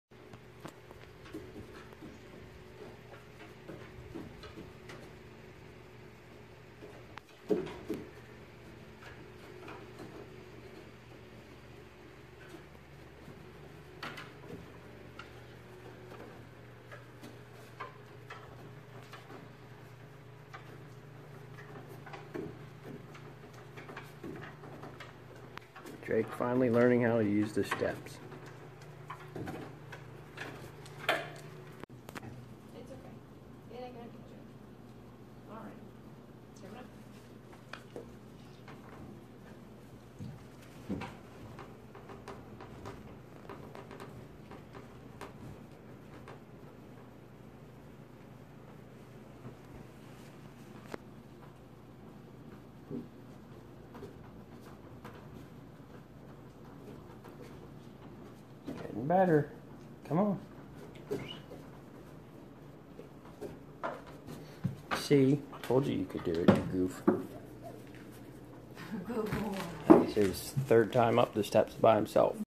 Our adopted great dane Drake learning to use steps.
Drake has never used steps until we got him. Slowly making progress. About 5 weeks since he lived with us.